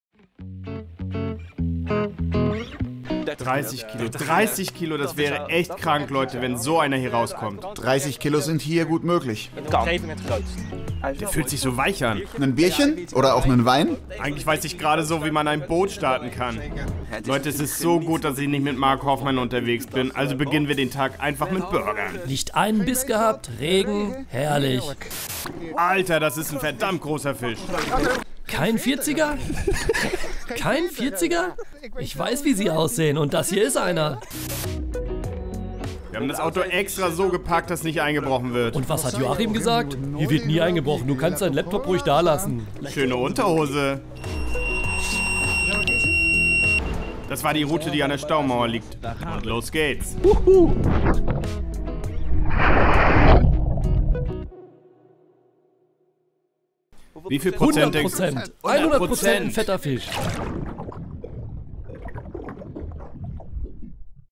Mission Big Flusskarpfen! Bootsangeln am River Lot
Karpfenangeln am wilden Fluss, dem River Lot in Frankreich. Die Schönheit dieses französischen Fluss ist unumstritten. Was könnte also spannender sein, als
dort auf Karpfen zu angeln? 30 Kilo Karpfen im Lot
, davon träumen die beiden
Holländer Michiel & Collin und
machen sich auf den Weg. Die beiden angeln von einem Boot aus
 und geben alles, um einen der großen Flusskarpfen zu fangen.

Es gelingt!
 Und tatsächlich fängt Colling
seinen Traumfisch.
...doch mehr dazu im neuen PRO-Film!